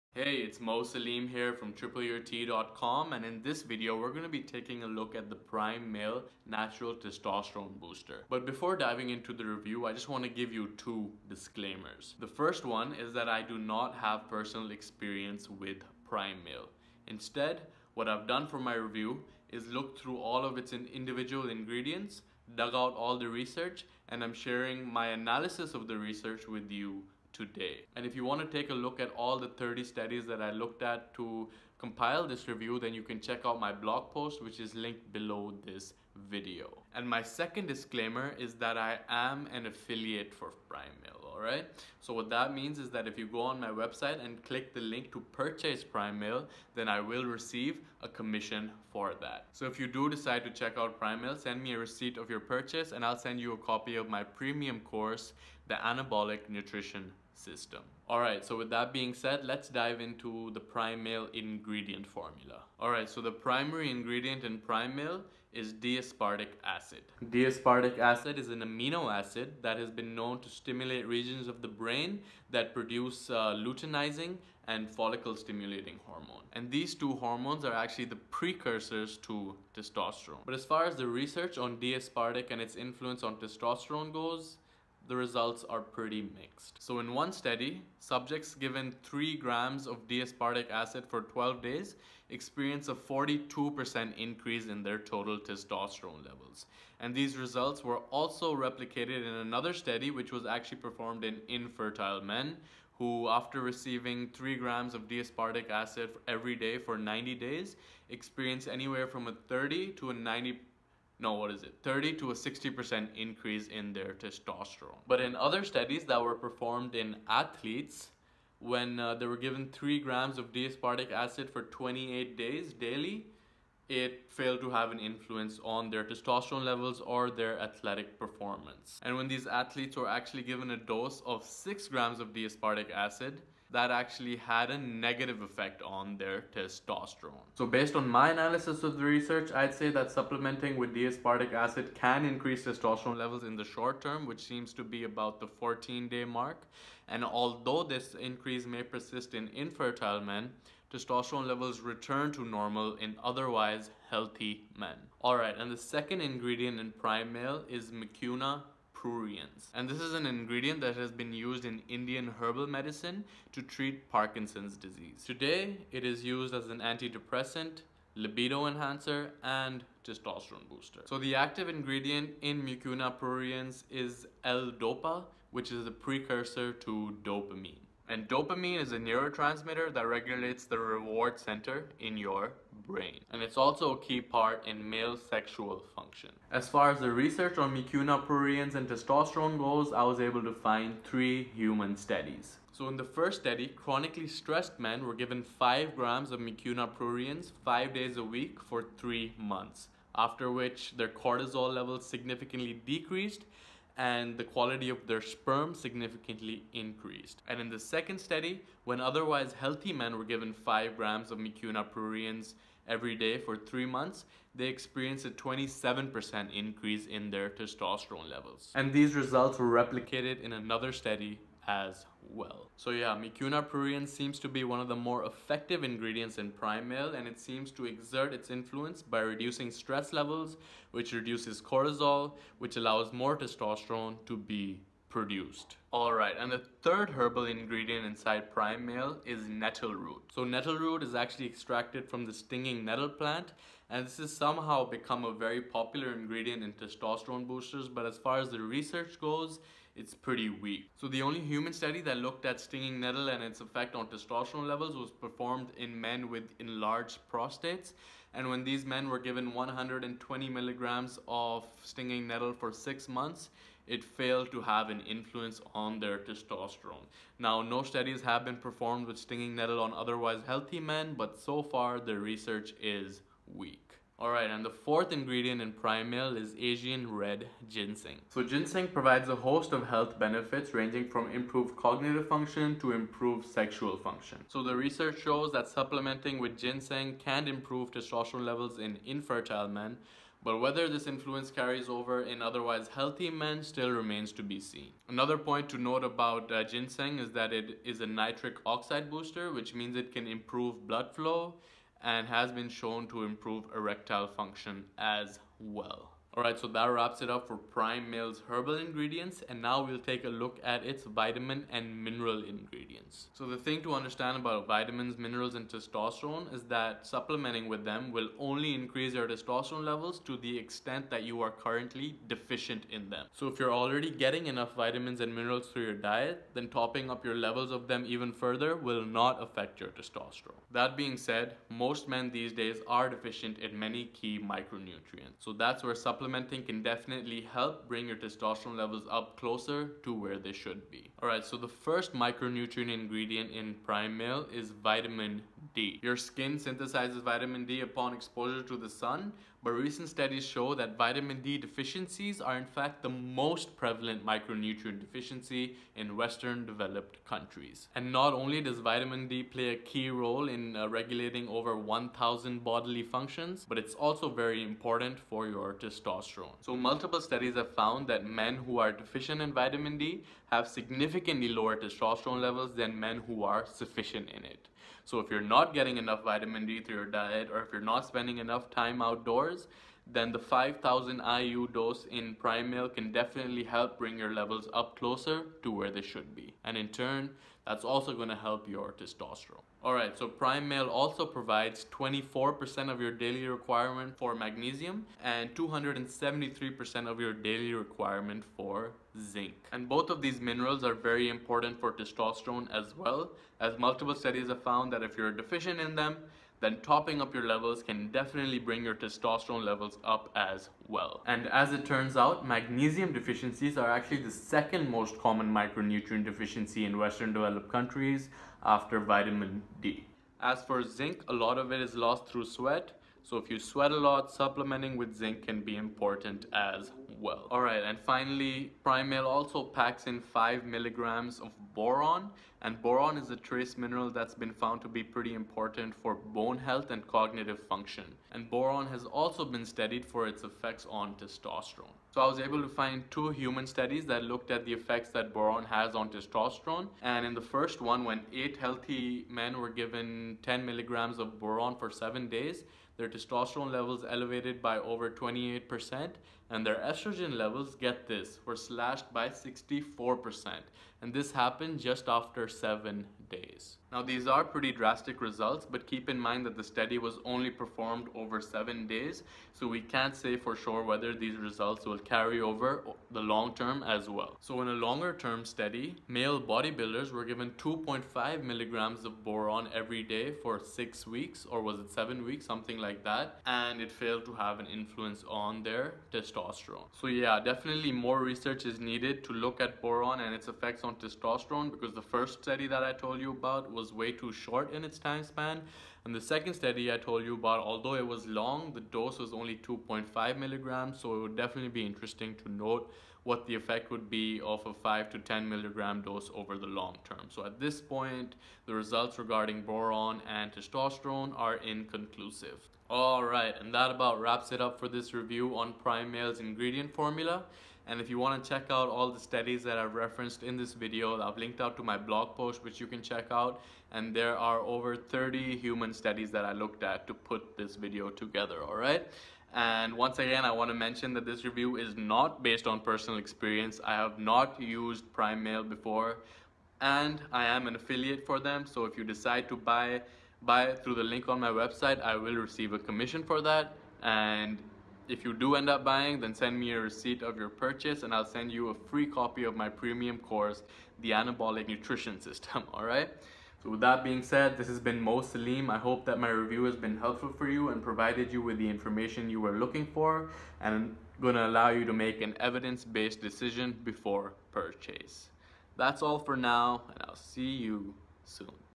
My Prime Male Review (Evidence-Based)
So you're thinking of trying out the Prime Male Testosterone Booster? My evidence-based review will reveal everything you need to know.

Hey guys, it's Mo Saleem here from Triple Your T and in today's video I'm gonna go over the research behind the Prime Male ingredients formula.

First, my review is not based on my prime male results. I have not used this supplement. Instead, what I've done is dug out the research behind each one of its ingredients. And second, I am an affiliate for Prime Male, which means that if you decide to purchase through the link on my site, I will receive a commission for that at no cost to you.

With that being said, here's my scientific review:

Ingredient #1 - D-aspartic Acid. D-AA is an amino acid that regulates the release of LH and FSH, which are the precursors for testosterone. Based on my analysis of the research, D-AA can boost testosterone levels in the short-term but levels return to normal within 28-days in otherwise healthy men.

Ingredient #2 - Mucuna Pruriens. Mucuna pruriens contains L-DOPA, which is the precursor for dopamine. Higher dopamine levels can improve sexual function. Studies show that supplementing with mucuna pruriens can increase T-levels by up to 26%.

Ingredient #3 - Nettle Root. This ingredient has not been shown to increase testosterone levels.

Ingredient #4 - Asian Red Ginseng. Ginseng is an NO booster which means that it can improve blood flow and erectile function. Studies also show that it can increase T in infertile men. Whether this effect carries over in otherwise healthy men remains to be seen.

Ingredient #5 - Vitamin D. Men with sufficient vitamin D levels have significantly higher testosterone compared to men who are deficient in it.

Ingredient #6 - Zinc. This mineral is also closely related to T-levels. If you're deficient in zinc, it's likely that your T is suffering.

Ingredient #7 - Magnesium. After vitamin D, magnesium deficiencies are the most prevalent micronutrient deficiency in developed countries. Topping up your magnesium, if you're deficient in it, can significantly boost your testosterone.

Ingredient #8 - Boron. Boron is a trace mineral which has some studies showing that it increased testosterone with others showing that it has no effect. More research is needed to confirm its true impact on testosterone.

Alright, so that wraps it up for this review.

Does Prime Male work? Is it a good bodybuilding supplement? Will it help you gain more muscle?

All I can say to answer these questions is: it depends. I've gone over the research and it's up to you to decide whether you want to try it out or not. I don't have any prime male before and after results because I have not used it.

My review is unbiased and was created to provide you with an inside look of what it actually contains.

To learn about how Prime Male stacks up against other test-boosters like testofuel and testotek, you can check out my complete blog post that I've linked out to above.